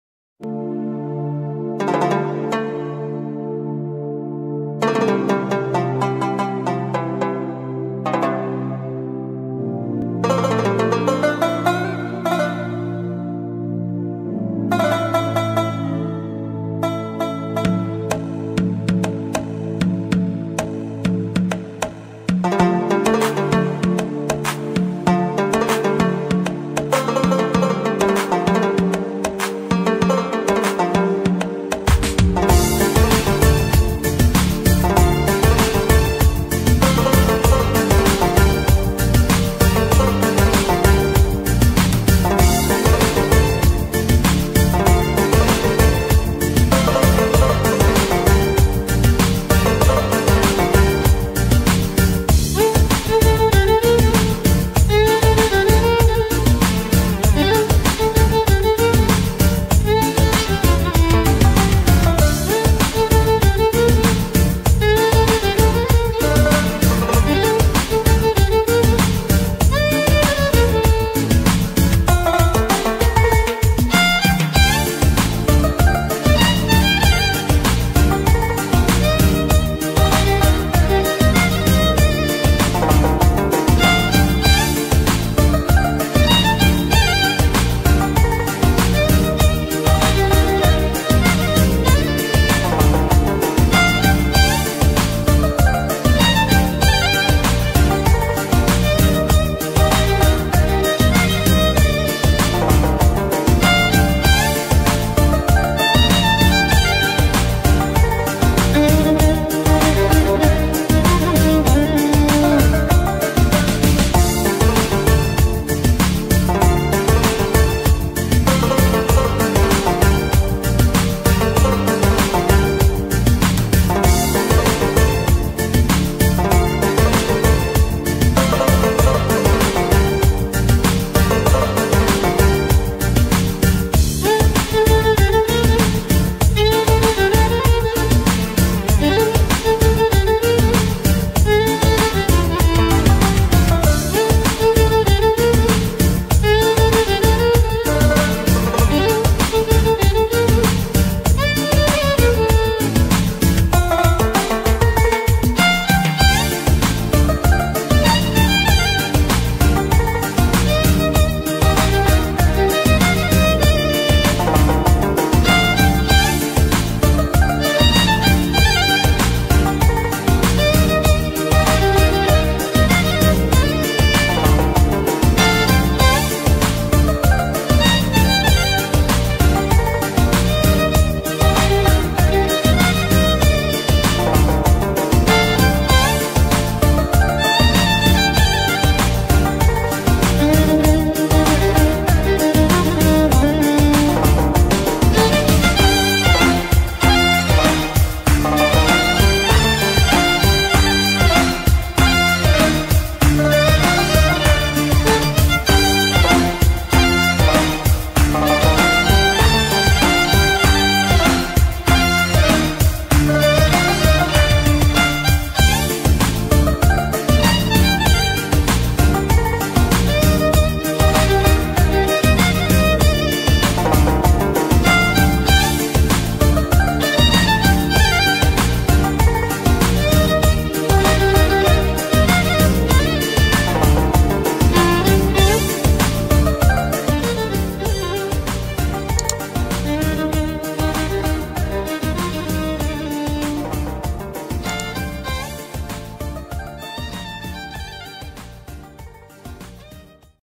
Slide Show with my travel motorcycle photo Adventure Traveller Greece motorcycle trips
Slide Show with my travel motorcycle photo from trips in Greece

Ένα όμορφο SlideShow που δημιούργησα με φωτογραφίες από φωτογραφίες από τις περιηγήσεις μου συνδιασμένες  με υπέροχες μουσικές.

Η μοτοσικλέτα που χρησιμοποιήθηκε είναι η Honda Transalp 600 cc.

Η λήψη του video έγινε με ActionCam : Gitup2 Pro και smartphone camera